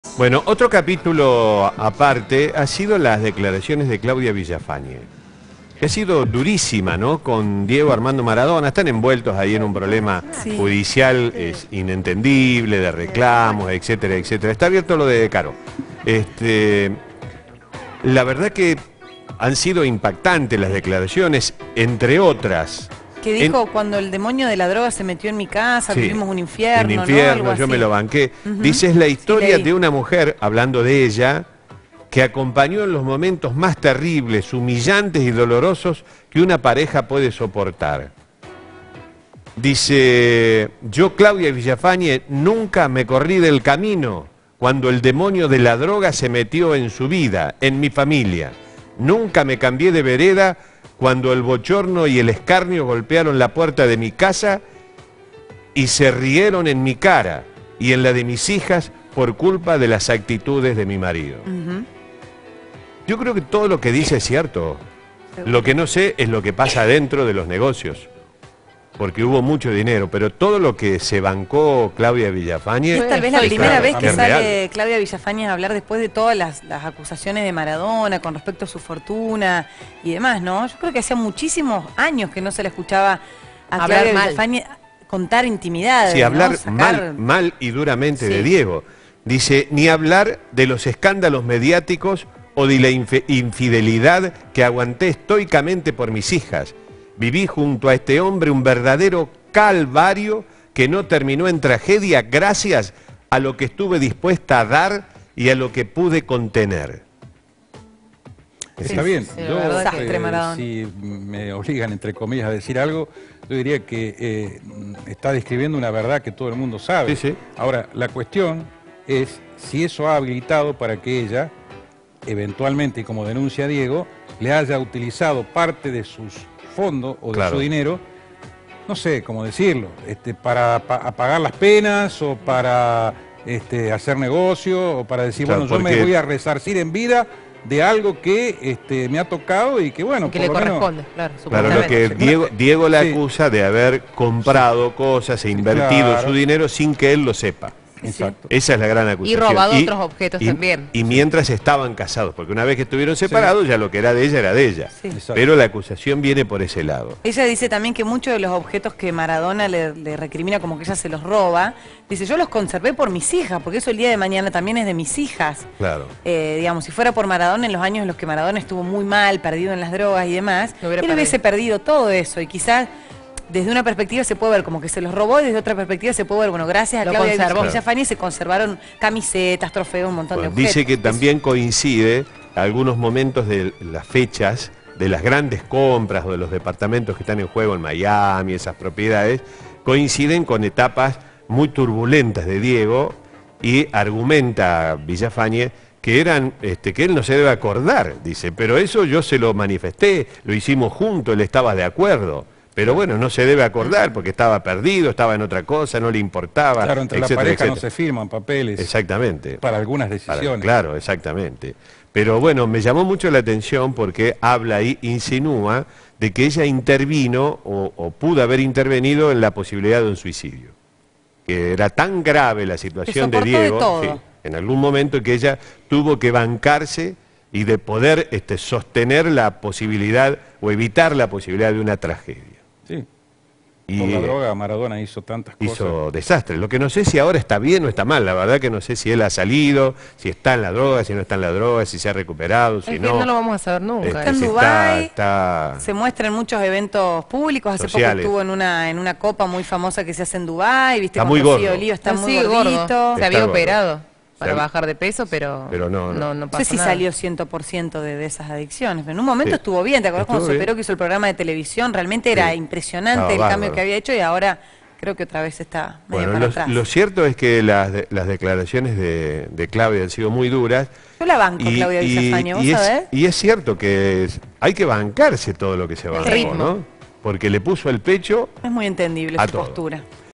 Duras declaraciones de Claudia Villafañe 04 04 2016 x264
Claudia Villafañe realizó duras declaraciones sobre su relación con Diego Armando Maradona. Por primera vez, Claudia habló de la forma en que los excesos de Maradona influyeron en su familia.